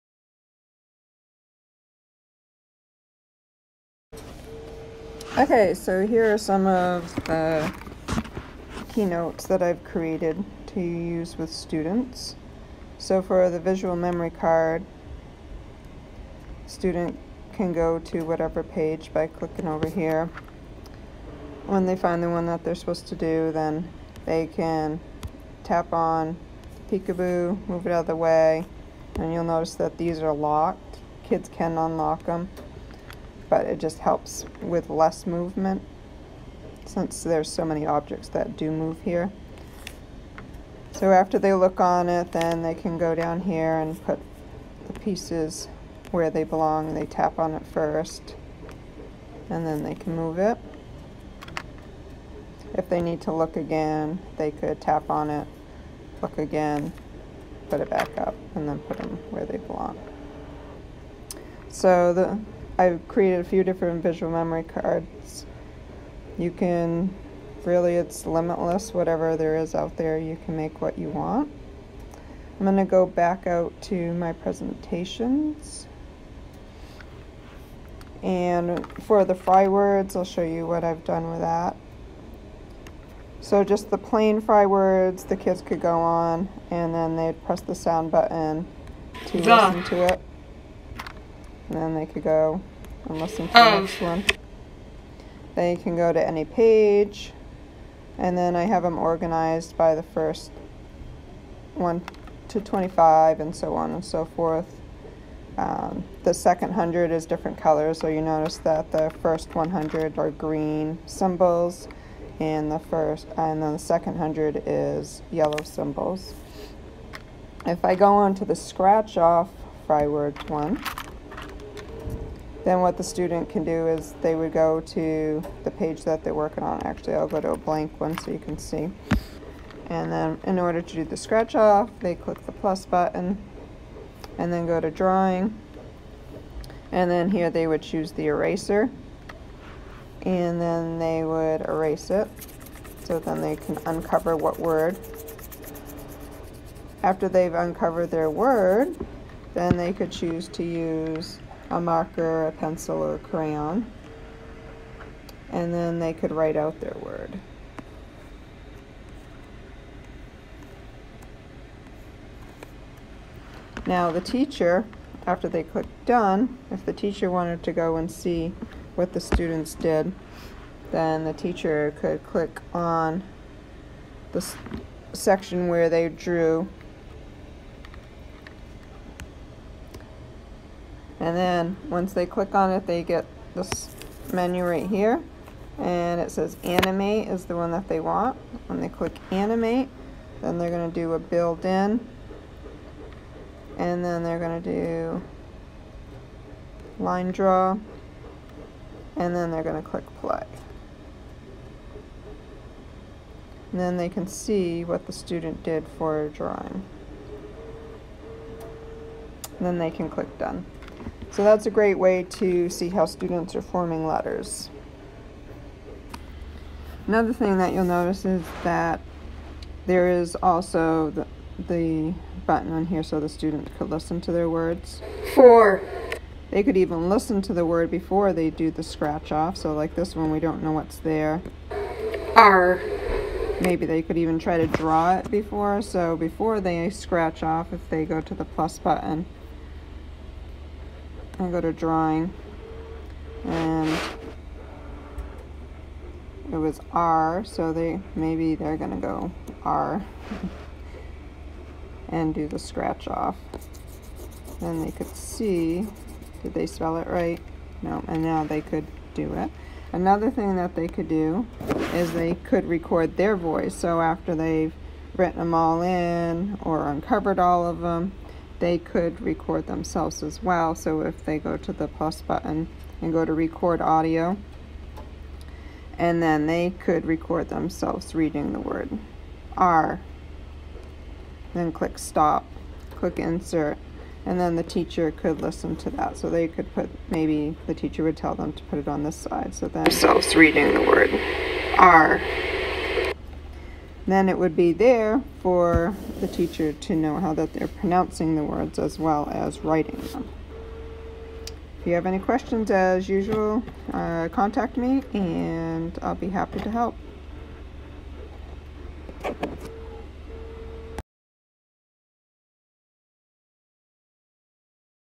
Uses for Keynote in the Elementary Classroom
Some ideas for for creating interactive slides with Keynote for students to use in the elementary classroom. Can be used on iPads or computers.